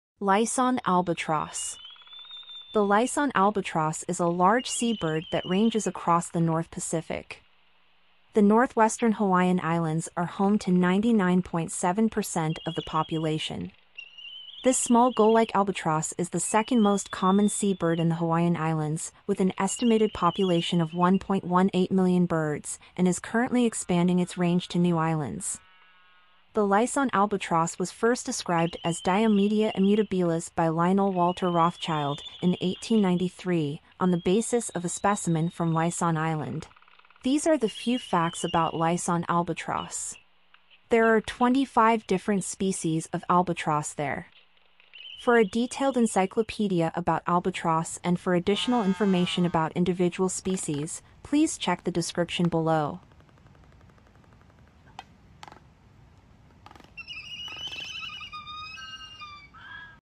Laysan Albatross - Video Encyclopedia
Albatrosses, of the biological family Diomedeidae, are large seabirds related to the procellariids, storm petrels, and diving petrels in the order Procellariiformes. They range widely in the Southern Ocean and the North Pacific. They are absent from the North Atlantic, although fossil remains show they once occurred there and occasional vagrants are found. Albatrosses are among the largest of flying birds, and species of the genus Diomedea have the longest wingspans of any extant birds, reaching up to 3.7 meter. The albatrosses are usually regarded as falling into four genera, but disagreement exists over the number of species.